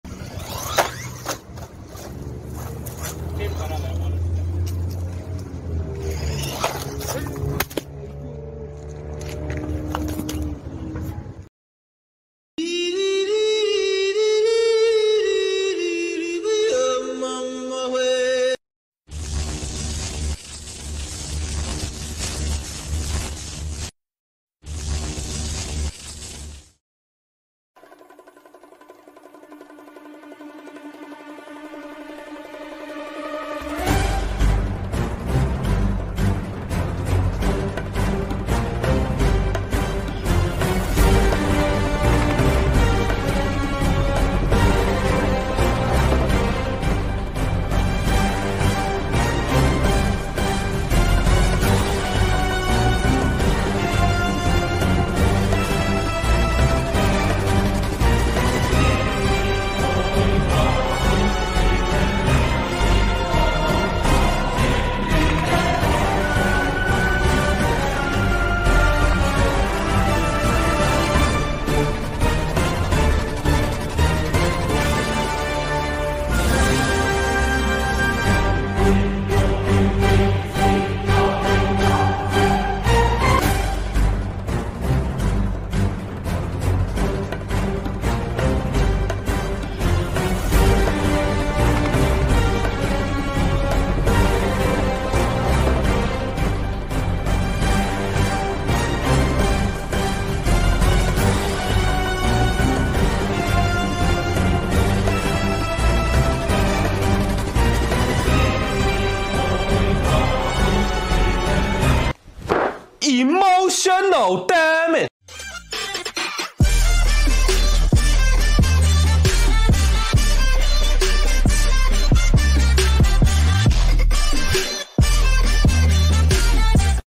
WLtoys A979-B Crazy 70km/h Monster Truck Bash - EMOTIONAL DAMAGE
This WLtoys A979-B, 1/18 scale monster truck is an upgraded version of the A979 with the following changes:
- Enormous 540 can size brushed motor instead of a 390 size
- Claims to run 70km/h instead of 50, which is absolutely crazy for a 1/18 scale car
- Metal differentials, cups and driveshafts
- 1500mAh, 7.4V LiPo with Deans / T-Plug connector instead of a JST one
- The wheels are secured with two stacked locking nuts to prevent them from becoming loose
- The biggest improvement is the remote controller with a much smaller steering dead zone around the center position! This change makes the car much better controllable. Very nice, WLtoys!

There are upgraded -B versions of the following WLtoys cars available: A959, A 969, A979

In my opinion, WLtoys has done a great job with the -B upgrade. I especially like the upgraded transmitter. It still looks the same, but is was a big surprise to see, that the steering is much better now!

Peace!

2) 2) Test Drive HSP Boxer 94706 4x4 Crawler (Back to the Journey) - DHS R/C Hobbies